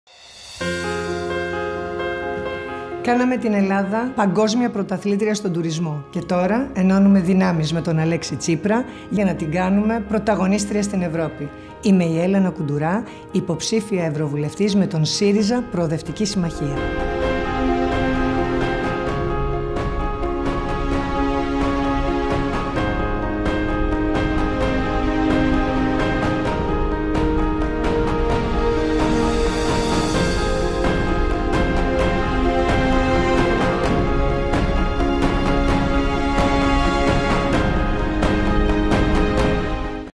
ΕΛΕΝΑ ΚΟΥΝΤΟΥΡΑ η καλύτερη Υπουργός Τουρισμού του κόσμου
Κάναμε την Ελλάδα παγκόσμια πρωταθλήτρια στον τουρισμό.
Στις 26 Μαίου ενώνουμε δυνάμεις με τον Αλέξη Τσίπρα και ψηφίζουμε για να κάνουμε την Ελλάδα των πολλών πρωταγωνίστρια στην Ευρώπη των λαών.